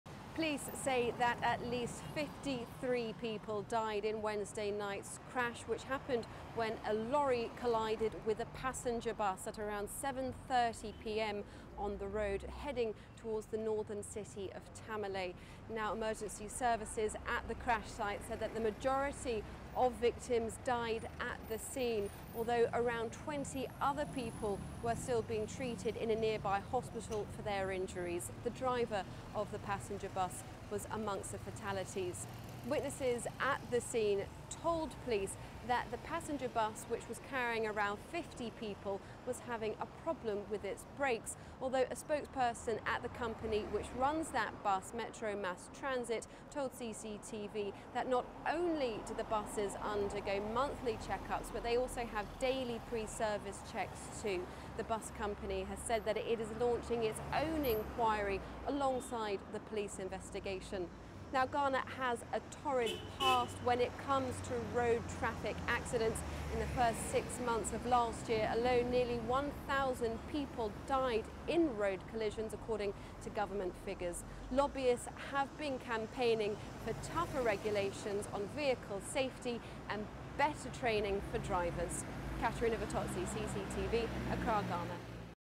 50 dead in grisly Ghana bus crash
At least 50 people were killed in a grisly accident involving a public service bus and a lorry.  The accident happened close to the town of Kintampo in northern Ghana on Wednesday evening.